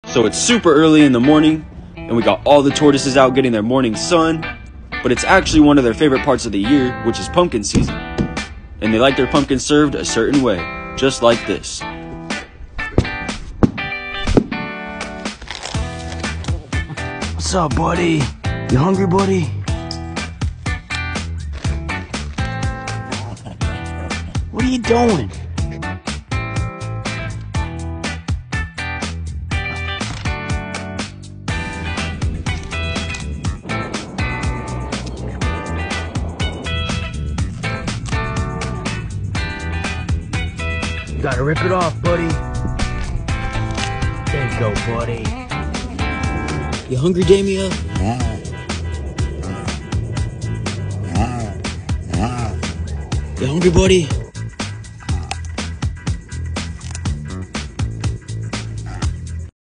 Tortoises LOVE pumpkin 🎃🐢
Smashing pumpkins for the tortoises 🎃🐢